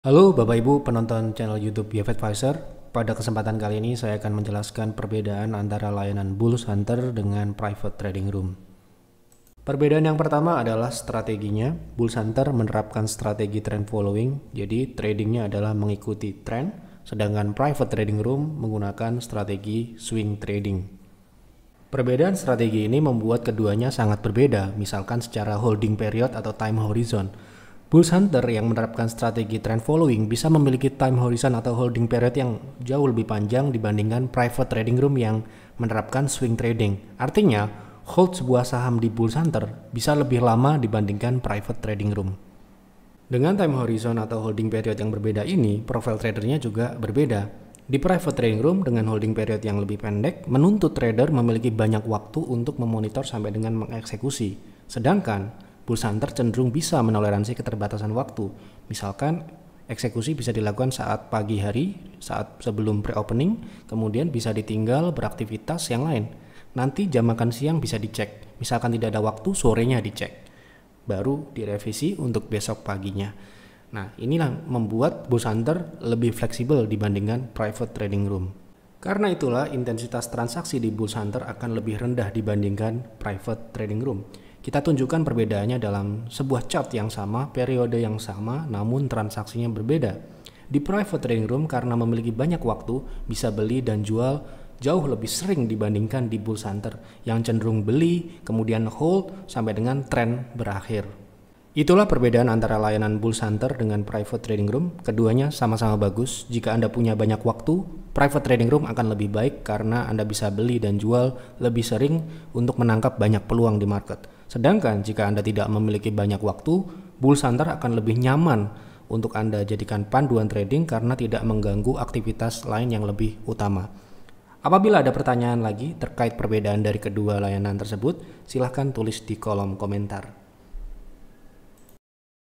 Perbedaan Bulls Hunter dengan Private Trading Room
Bulls Hunter & Private Trading Room kami buat untuk melayani profil trader yang berbeda oleh karenanya keduanya memiliki perbedaan yang signifikan. Dibalik perbedaan tersebut keduanya sama sama layanan yang baik, dibuat dengan Kesungguhan dan ketelitian yang tinggi!

Layanan Bulls Hunter akan resmi dimulai pada tanggal 3 Juni 2024 & pendaftaran member dimulai pada tanggal 5 Mei 2024. Pendaftaran bisa melalui admin di WA & Telegram

Berikut ini langkah pendaftarannya

Ketik format pendaftaran
Layanan/Paket: bullshunter/(3/6/12)
Nama Lengkap:
Domisili:
No WA :

Apabila ada pertanyaan terkait Bulls Hunter jangan segan untuk berkomentar ya

Terima kasih

(((Waspada & Hati-hati Penipuan!)))

Kami tidak terafiliasi dengan pihak manapun & Tidak menerima TITIP DANA!

-YEF & Team